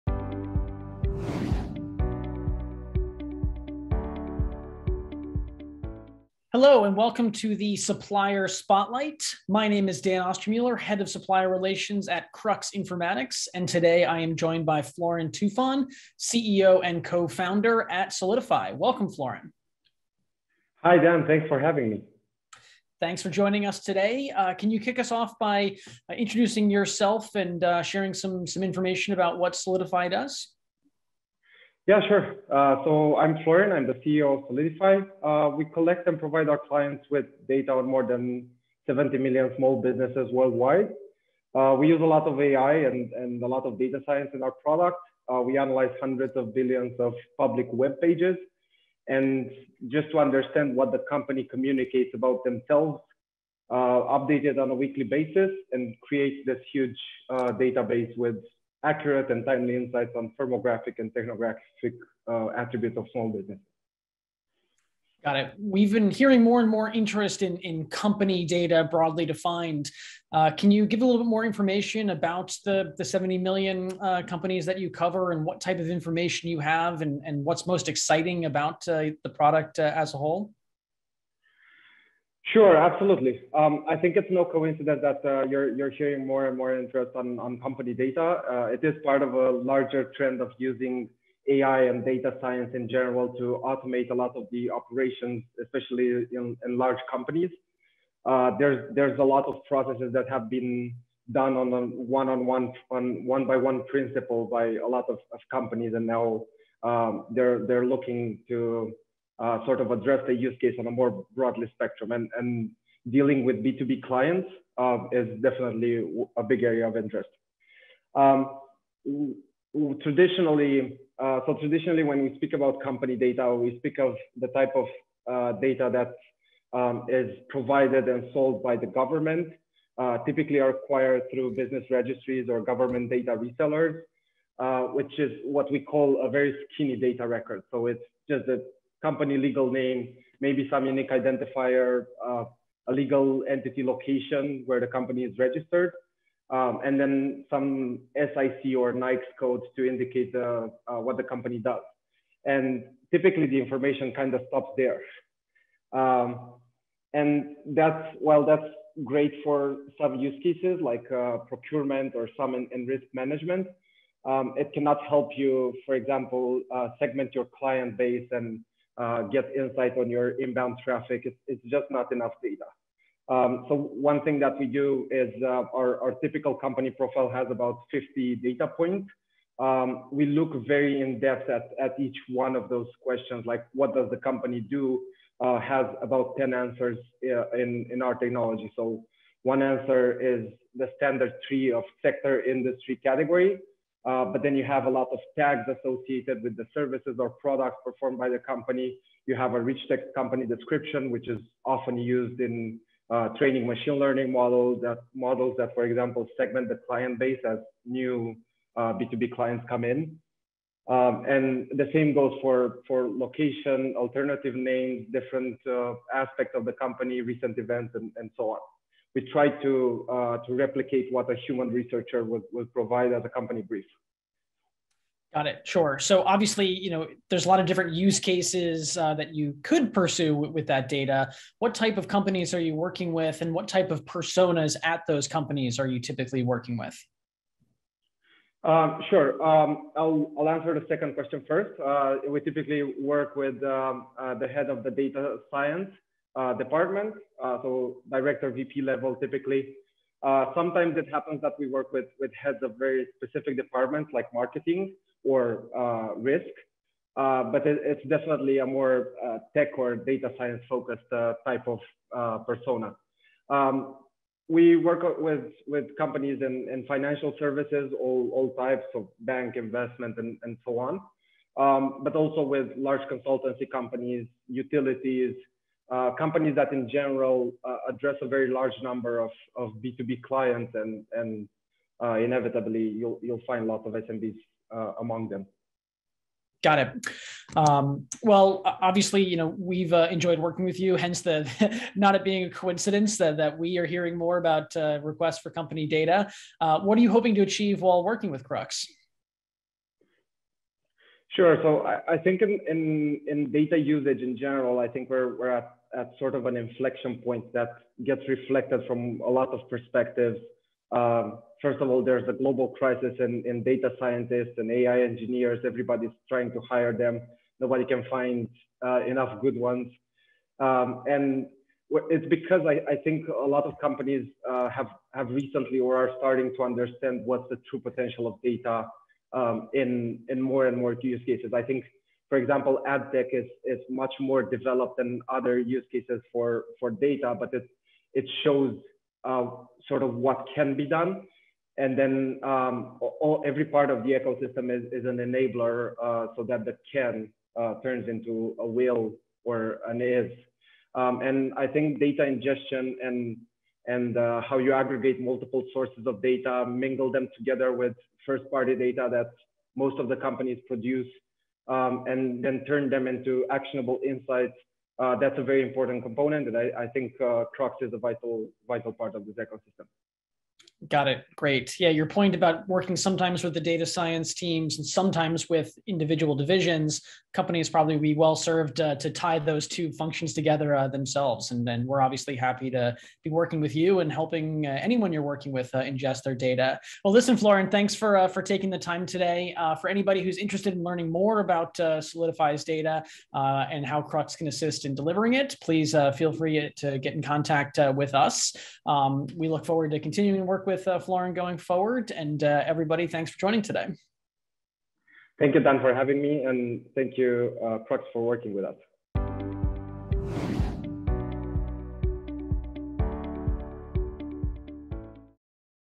Crux Supplier Spotlight: Soleadify
Soleadify provides business data enrichment with unprecedented coverage of private companies, accurate classification and in-depth insights based on real-time updates, for procurement, insurance, market intelligence and more.